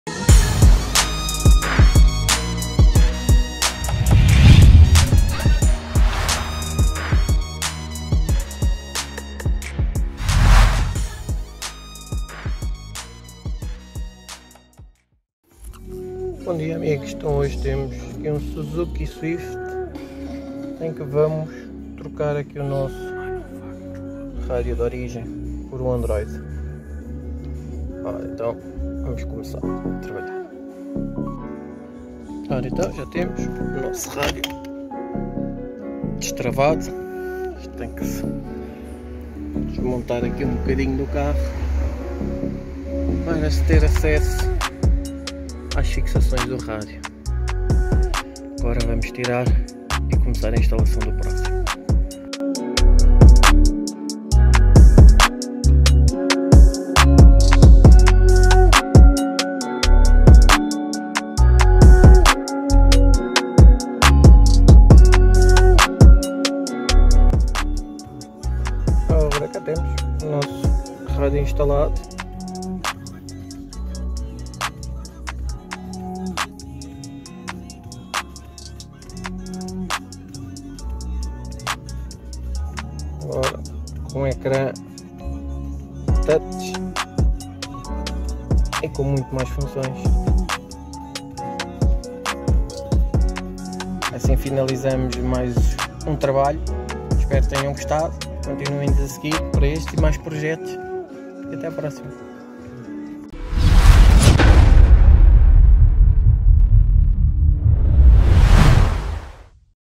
Suzuki Swift Radio Upgrade
Neste video fazemos um upgrade no radio deste Suzuki Swift...